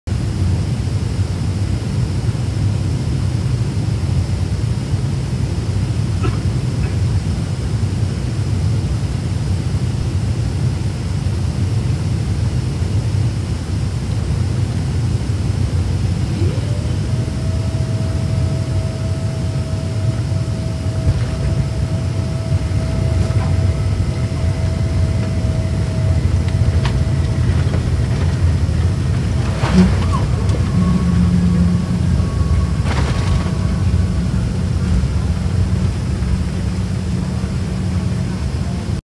Airplane Landed in Real Life
Plane is landing at night